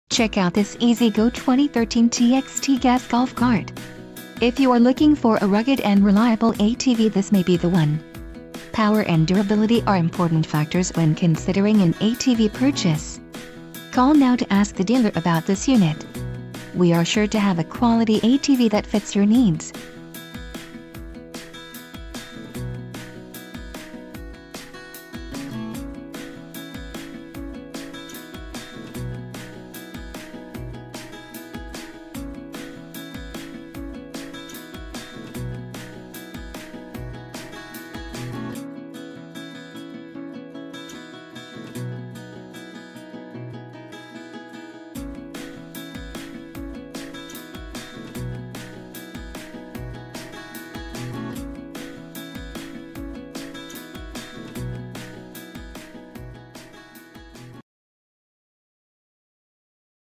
2013 E-Z-Go TXT Gas Golf Cart
EZ-GO TXT 13hp 4 Passenger Gas Golf Cart.
FEATURES:

 13 hp Kawasaki Engine
 New Blue/White Body
 Tan Seats
 New Rear Flip Seat
 Tan Sun Top
 Clear Folding Windshield
 New LED Headlights
 New LED Tail Lights
 8" Wheels
 18x8.5-8 Tires
 Ez-Go Wheel Covers

Great Running Low Hours Northern Cart!